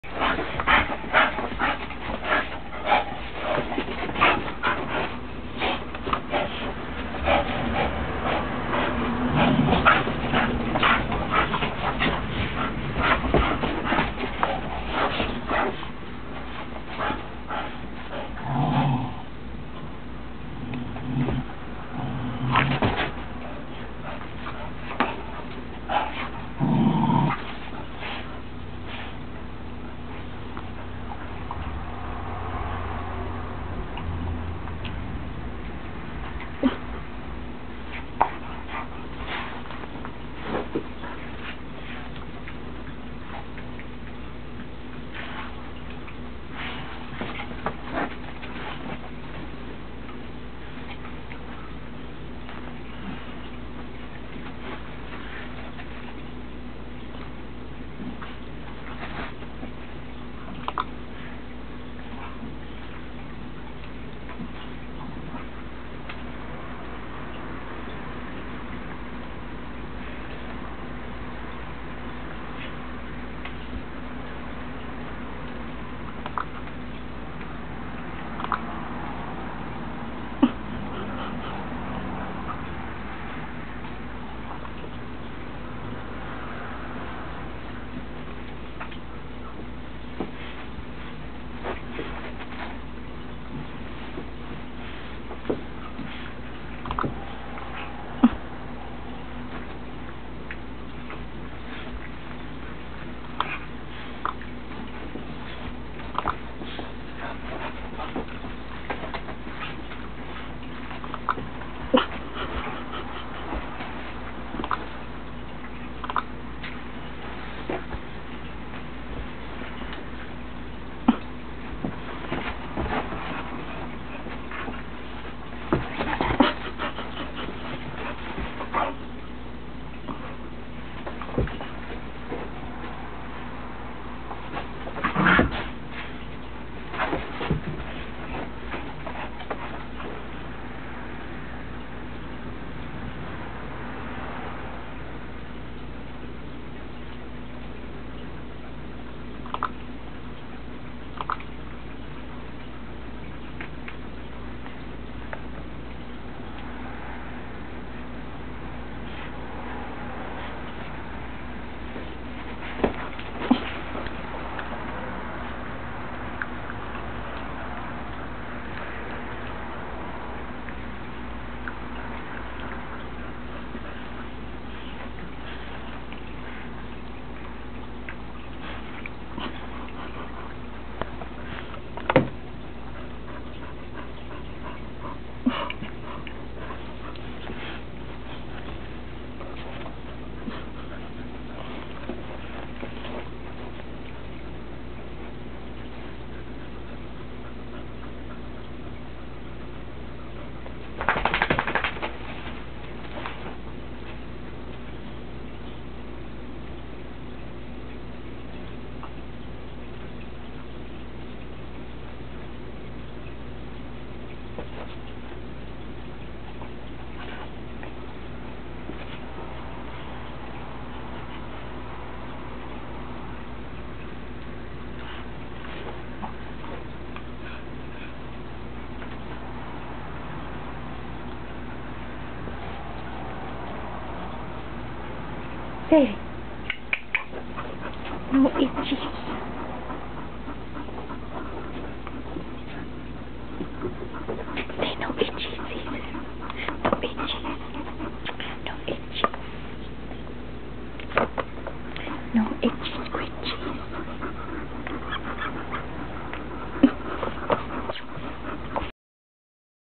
Saddie and Cesar
saddie and Cesar with a rope ball. as you can see saddie is a little possessive of toys, we are working on that